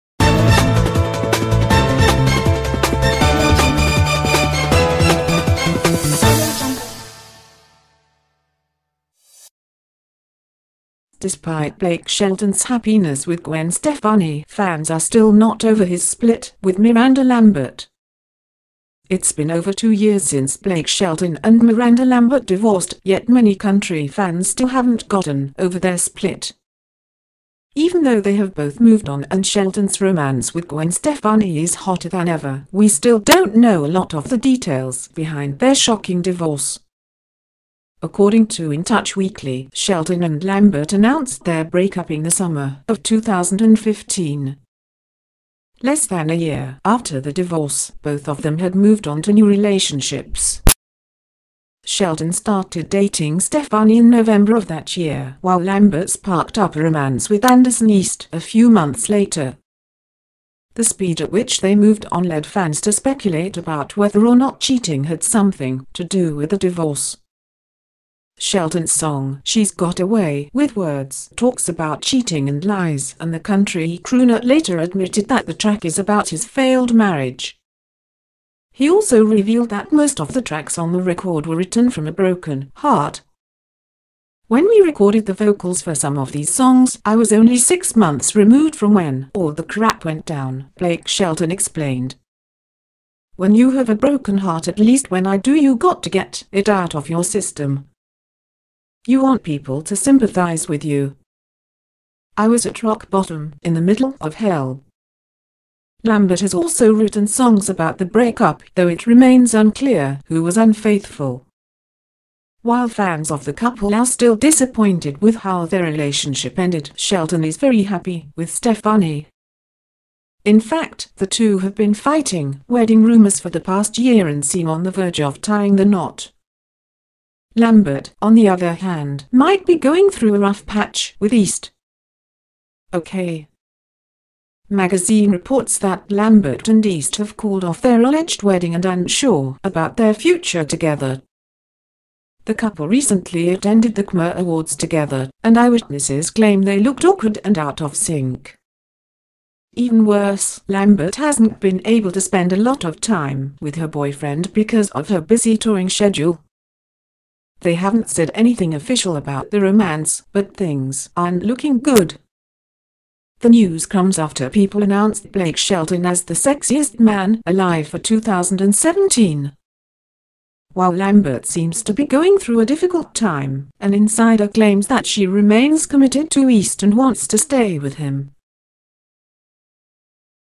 Despite Blake Shelton’s happiness with Gwen Stefani, fans are still not over his split with Miranda
It’s been over two years since Blake Shelton and Miranda Lambert divorced, yet many country fans still haven’t gotten over their split. Even though they have both moved on and Shelton’s romance with Gwen Stefani is hotter than ever, we still don’t know a lot of the details behind their shocking divorce.